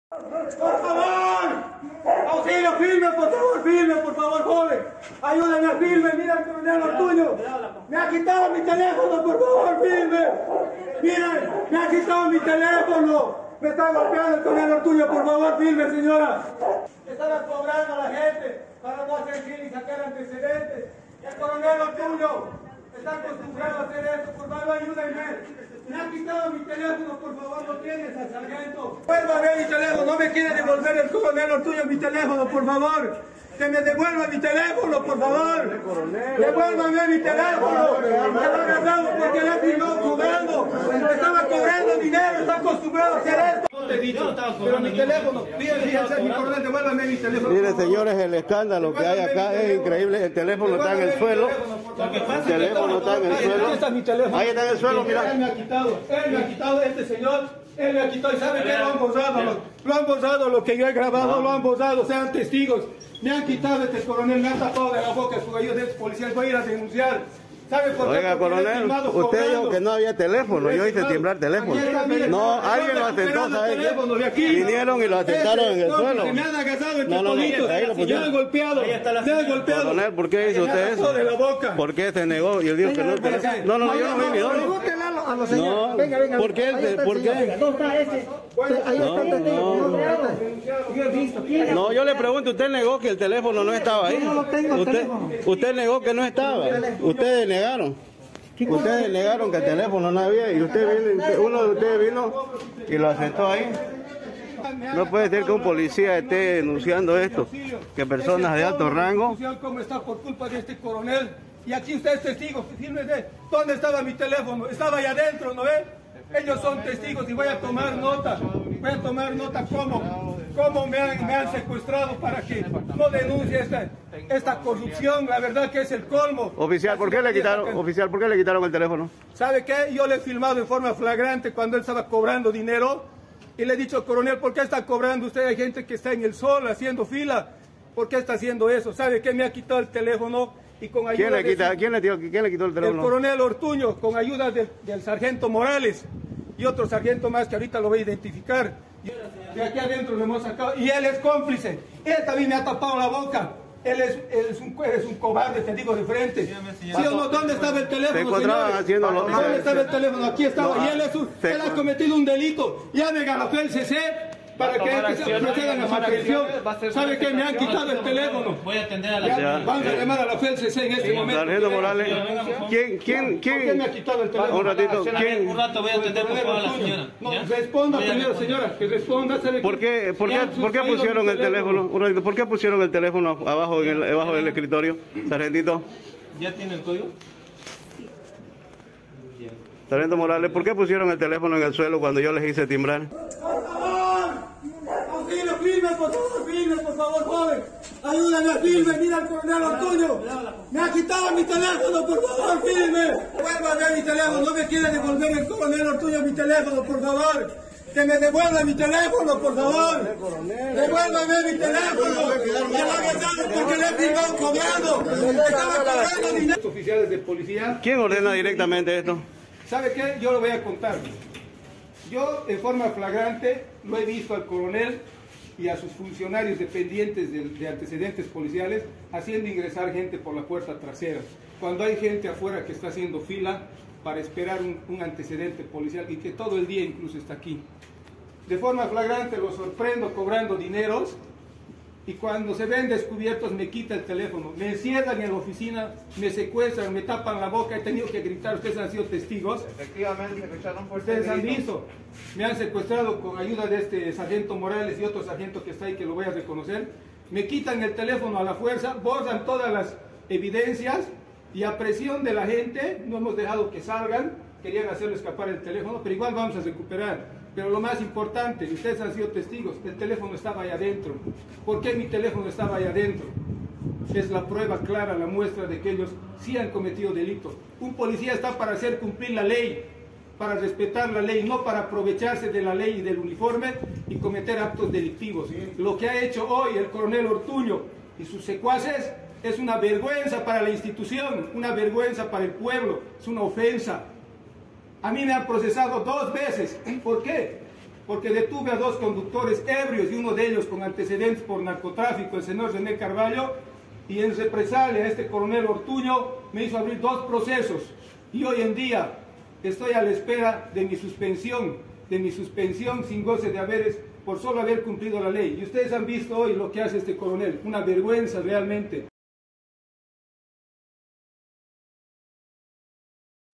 Policía denuncia corrupción e intento de acallarlo, en recaudación aparentemente se cobraba por real
Policía denuncia corrupción e intento de acallarlo, en recaudación aparentemente se cobraba por realizar trámite agilizado por un coronel y subalternos